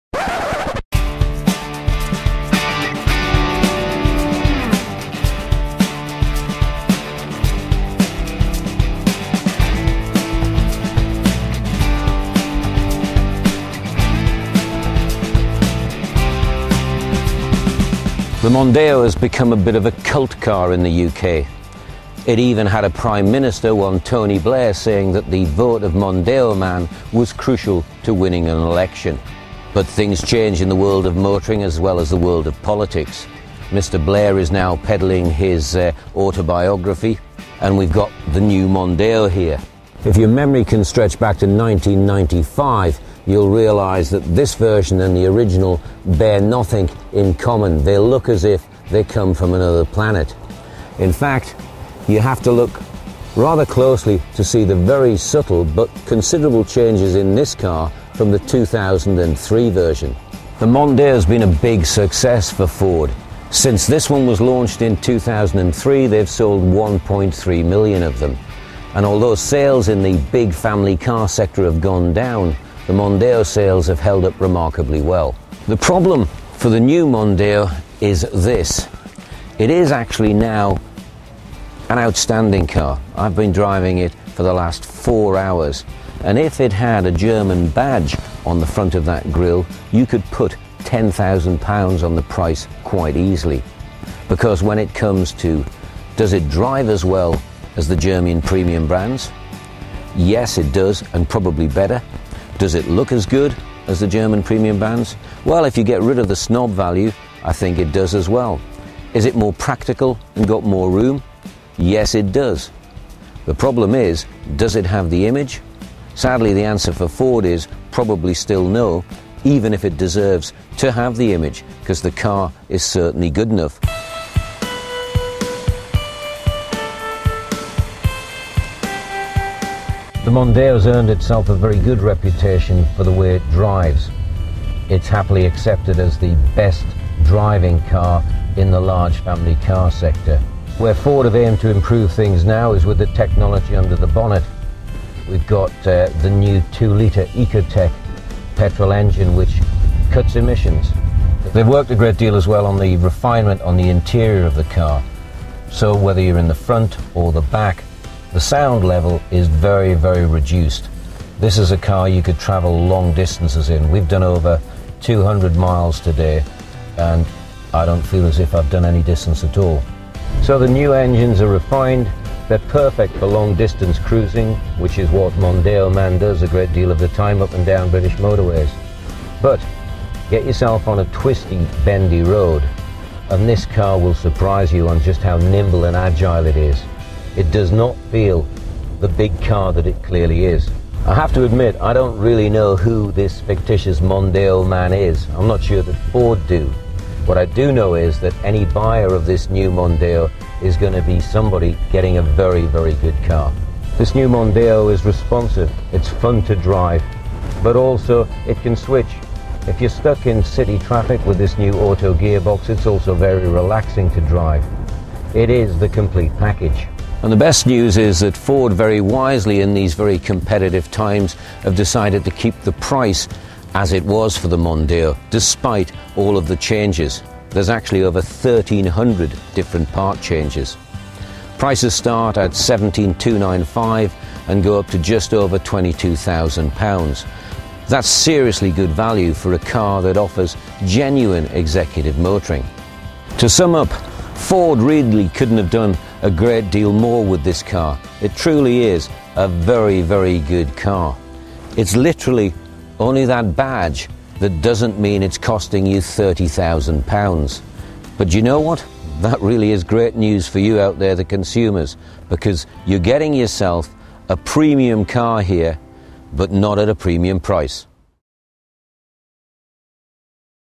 Mondeo Man gets promoted
Ken test drives the latest version of Ford's big family car